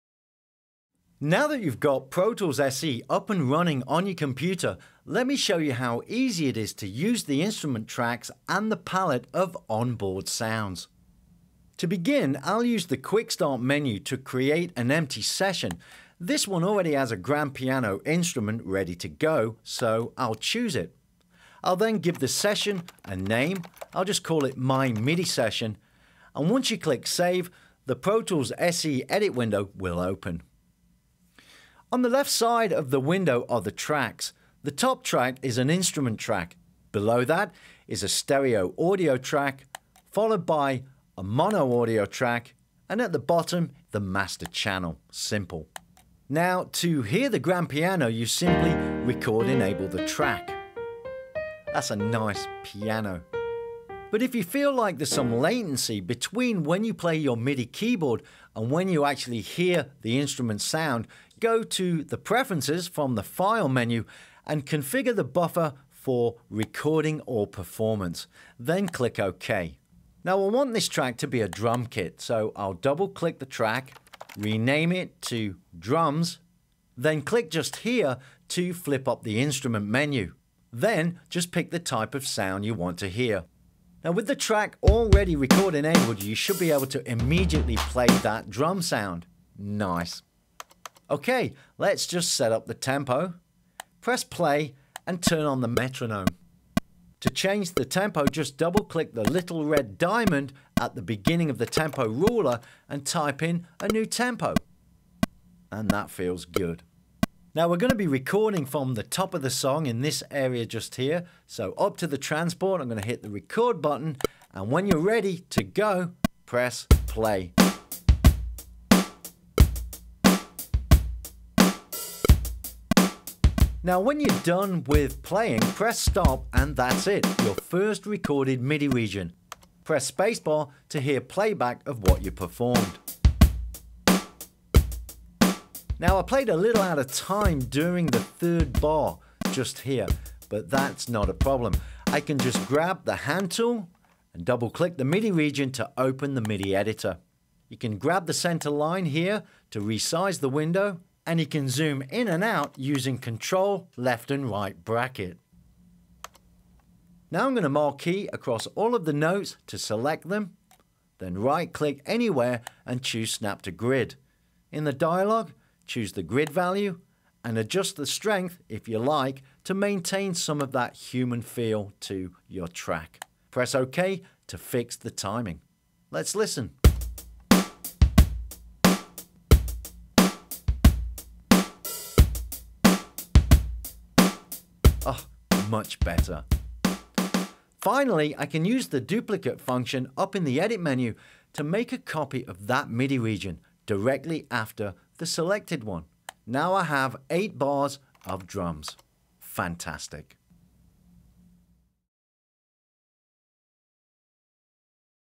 Pro Tools® SE - Recording an Instrument Track - Win 7 & Mac OS X
This video shows you how easy it is to record an Instrument Track (MIDI Track), in Pro Tools SE. The video also covers Quantizing, or correcting the timing of a performance.  Please watch at 720p for best picture quality.

SUGGESTED LINKS OF INTEREST